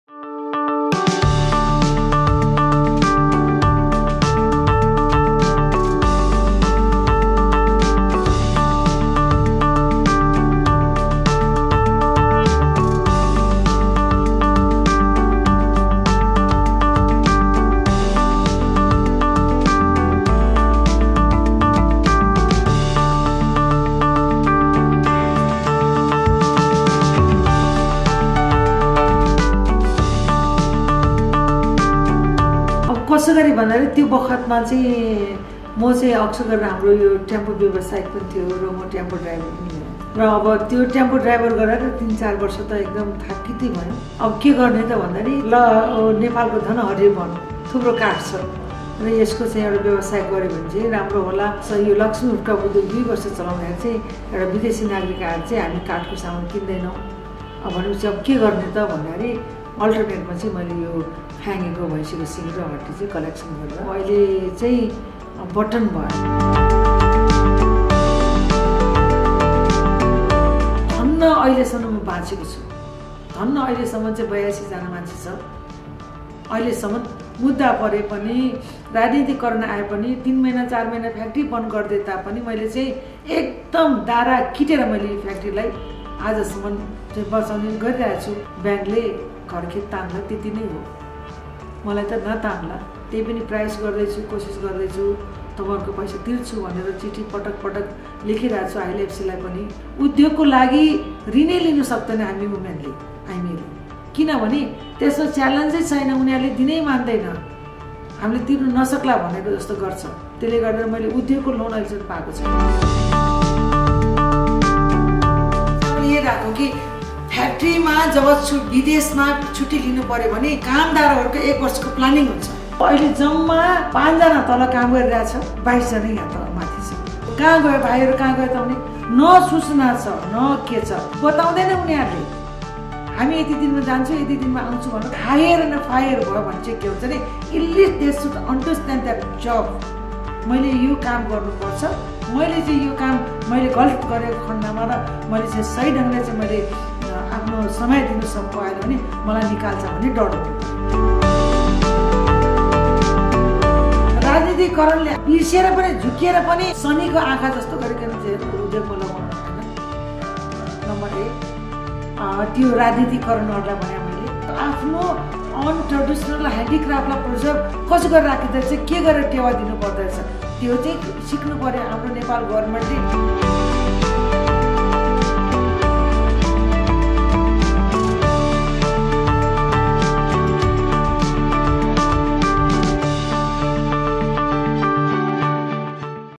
Laxmi Sharma from Laxmi Wood Craft Udhyog
Laxmi Sharma owns and runs ‘Laxmi Wood Craft Udhyog’, the premier exporter of quality buttons to fashion houses like Ralph Lauren and Zara among others.